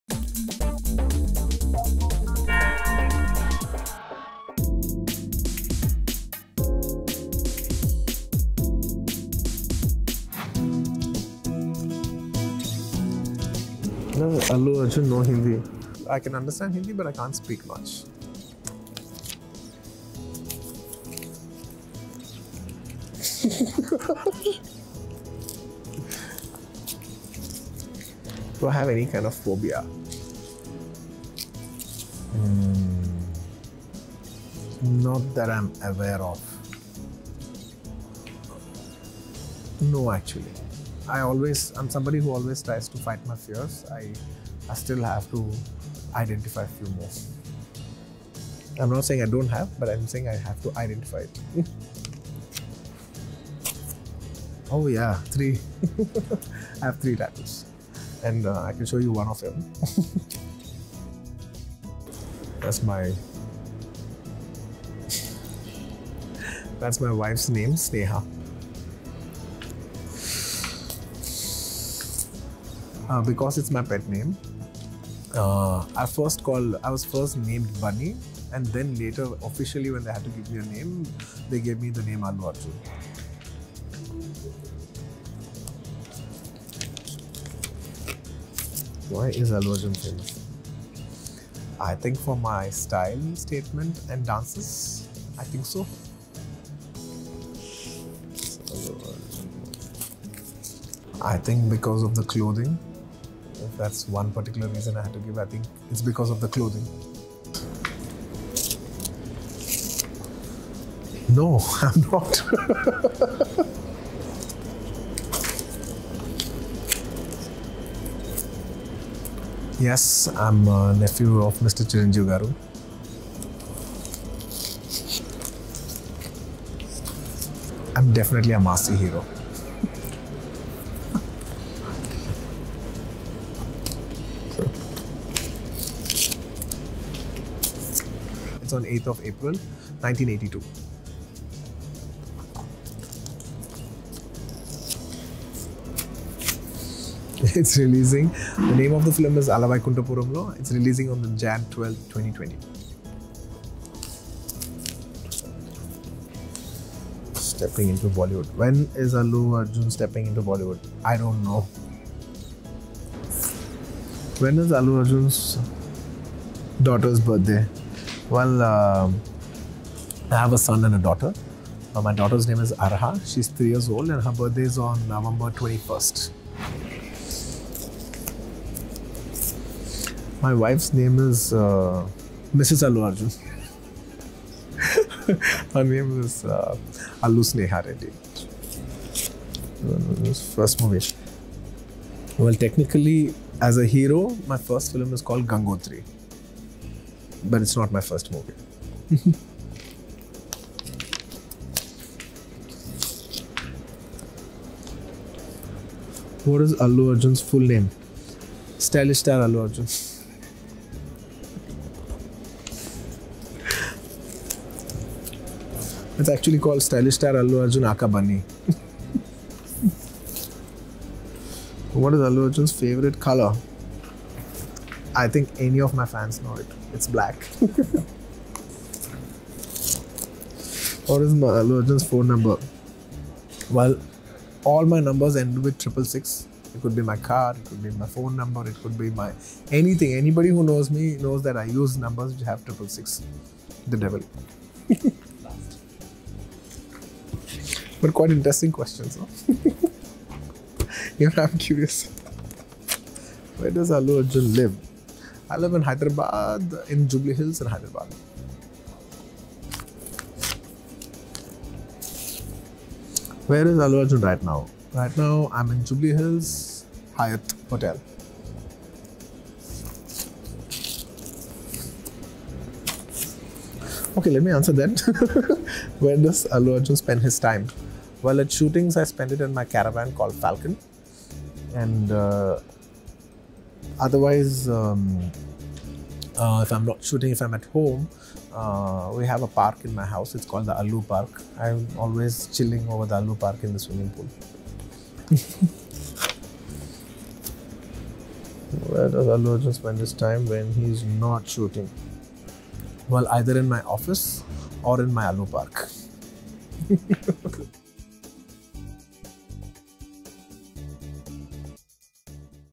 Allu Arjun answers Google's most searched questions in his
Stylish star Allu Arjun answered the most Google-searched questions on him in our special segment.